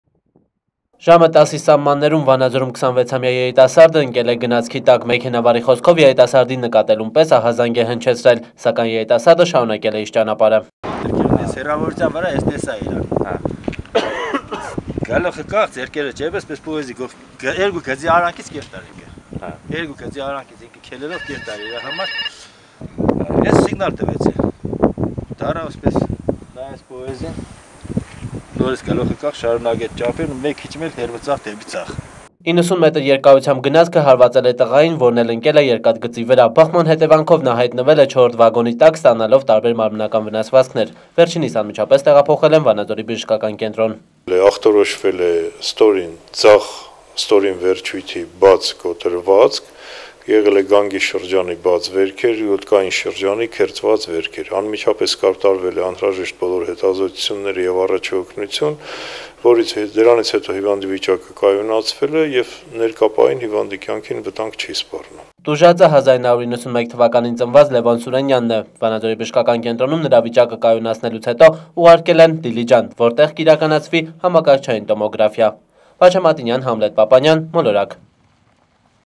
Լոռու մարզում գնացքի տակ ընկած երիտասարդը ողջ է մնացել...
Նոյեմբերի 23-ին ժամը 10-ի սահմաններում Վանաձորում 26 ամյա երիտասարդը ընկել է գնացքի տակ: Մեքենավարի խոսքով երիտասարդին նկատելուն պես ահազանգ է հնչեցրել սակայն երիտասարդը  շարունակել է իր ճանապարհը: